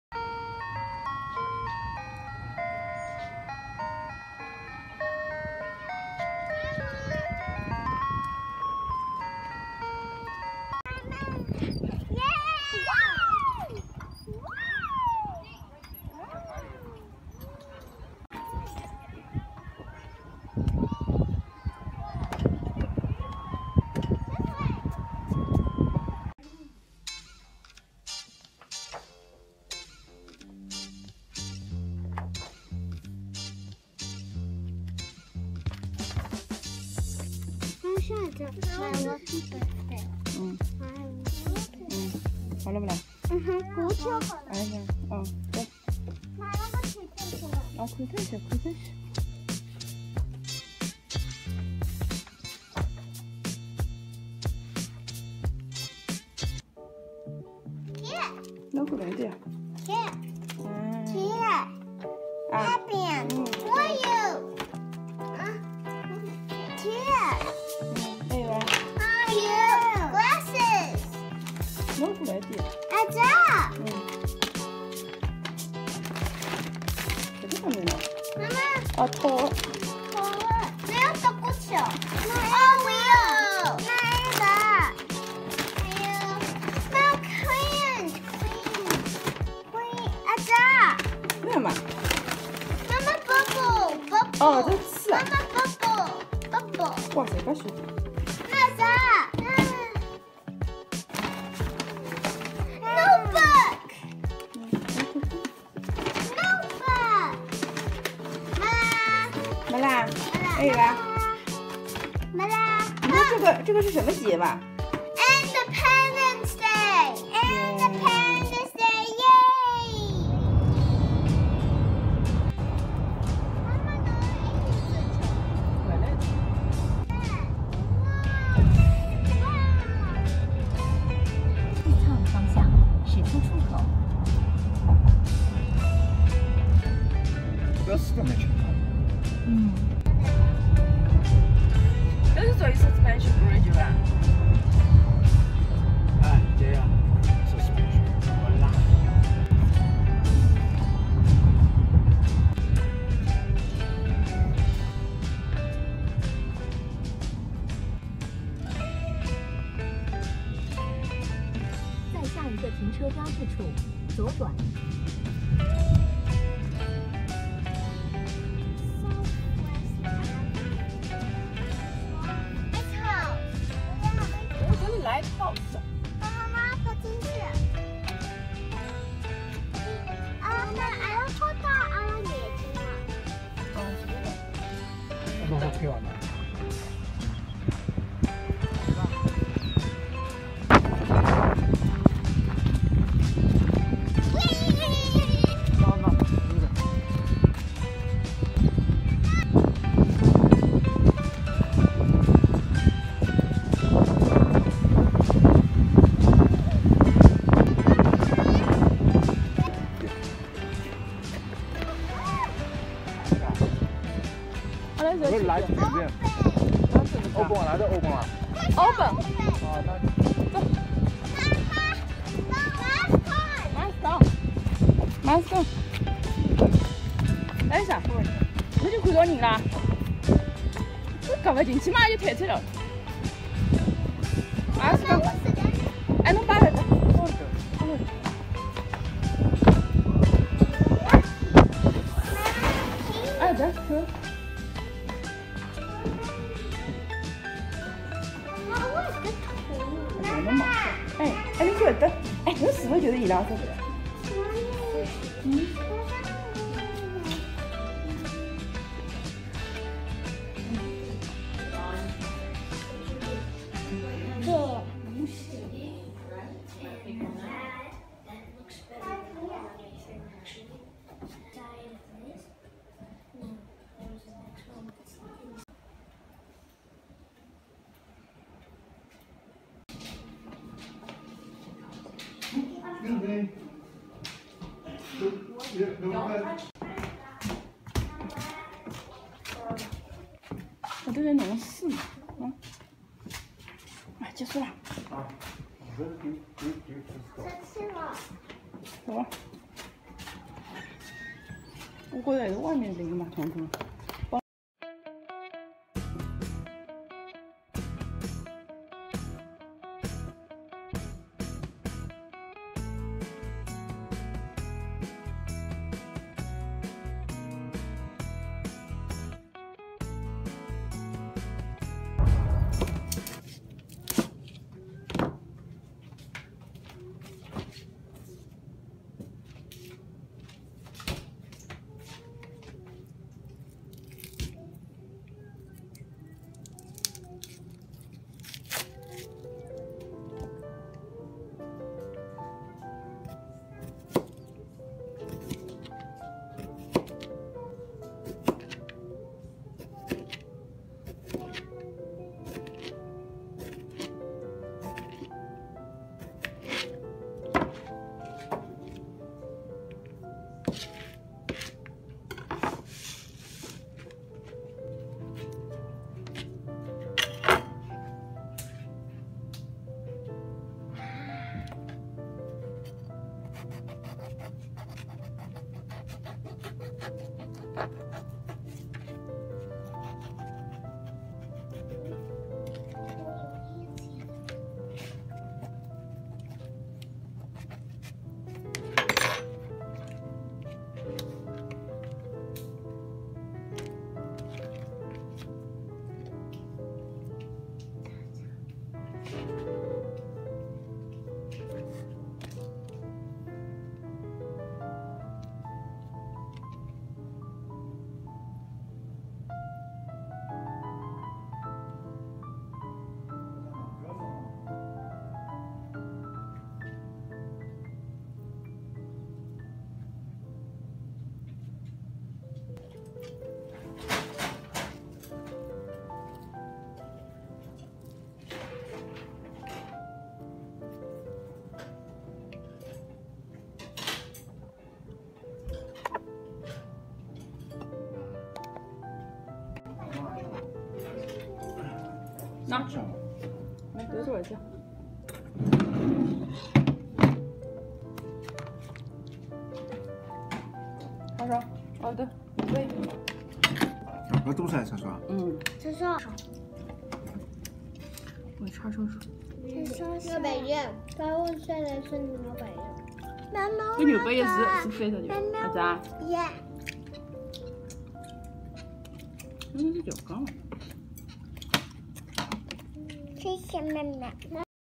暑假VLOG#1 | 开箱独立日礼物 | 吃最好吃的叉烧可颂三明治 | 访第三大古老灯塔
Hi~大家好呀~暑假开始了，今天分享的是假期点滴。